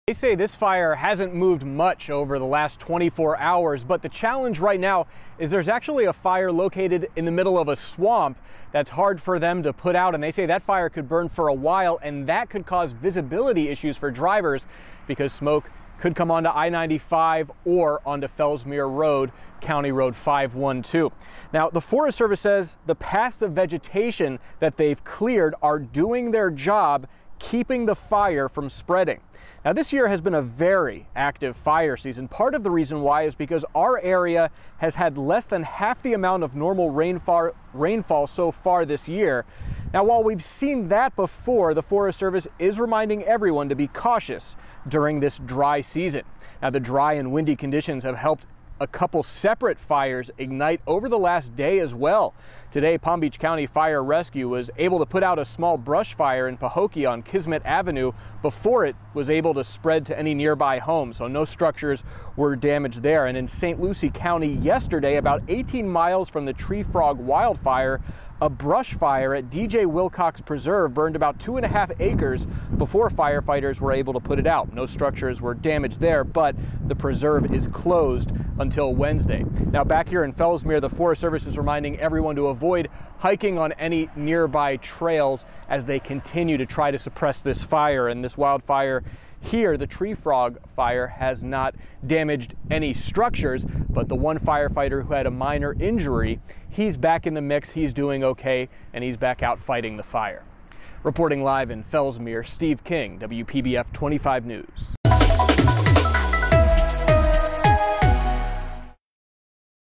Tree Frog Wildfire Update
A wildfire continues to burn in northern Indian River County. WPBF's Steve King has the latest.